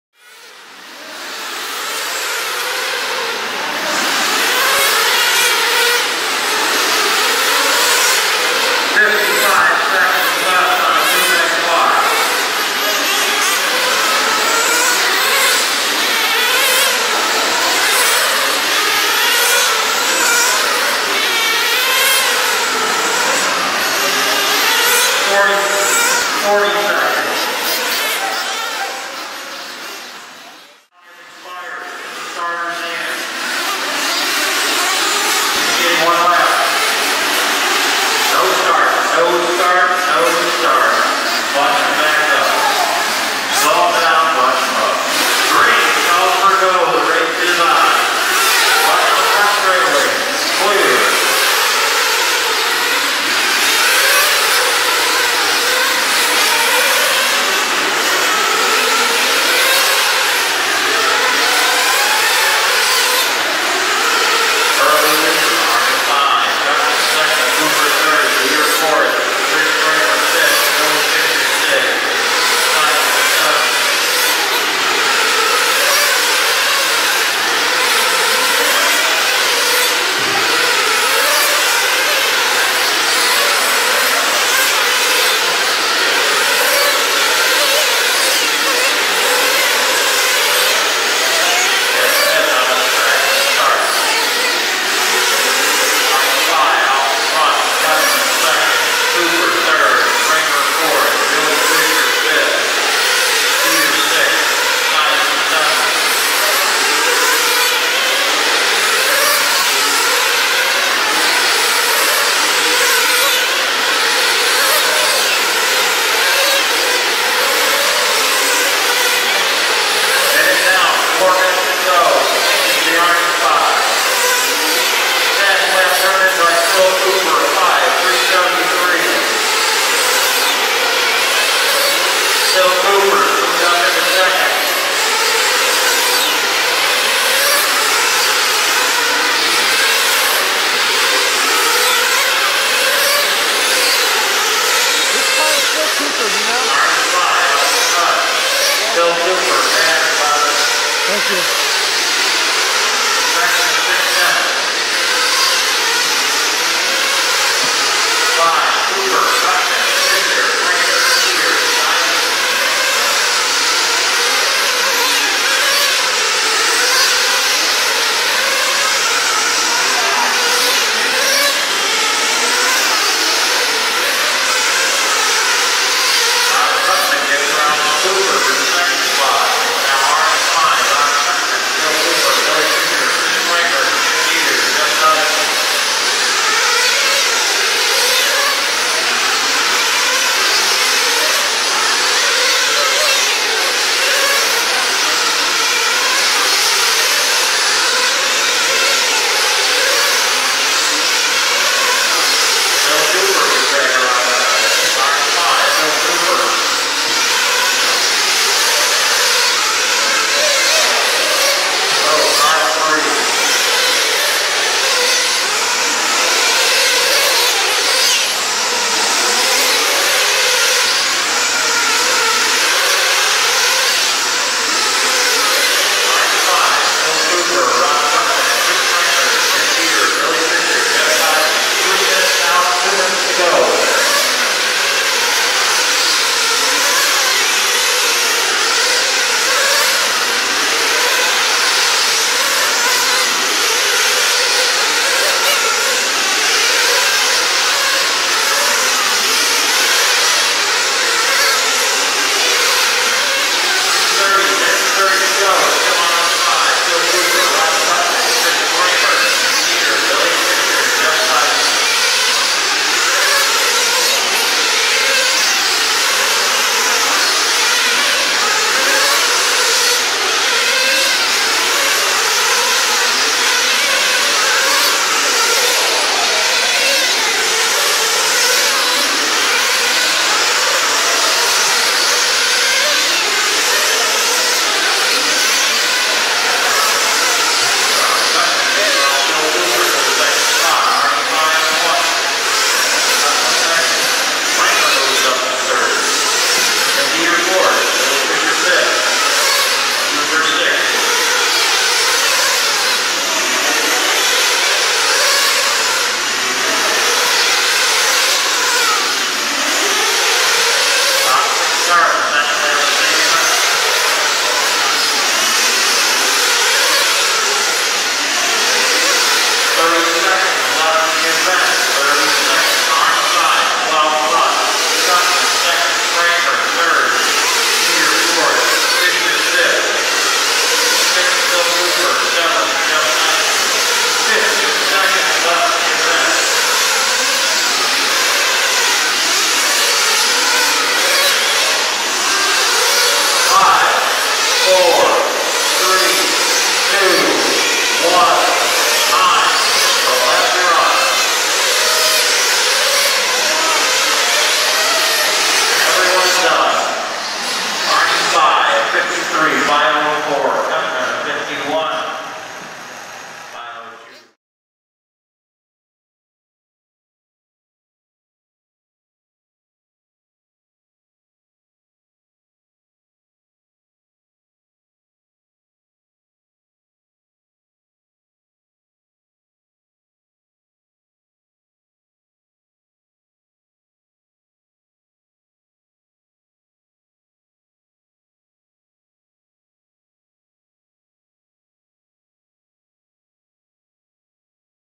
MONEE 2005 DODC Nitro Tour National Race – Sprint A
Monee RC Raceway in Monee, IL. October 22nd & 23rd 2005 Fall Round Up.
- Sprint A-Main
- Produced By RC OvalAction.com